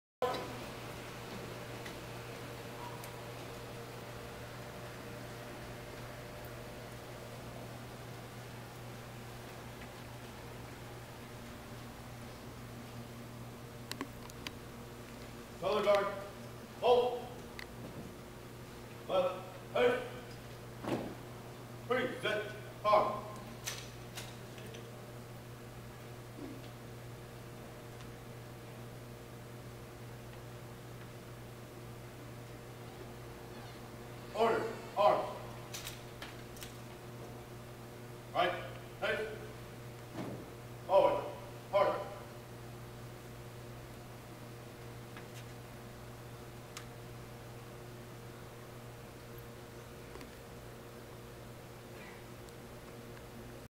Volunteer of the Year - 2009 - Color Guard
Thanks to Mainland Highschool's ROTC for the presentation of the flags.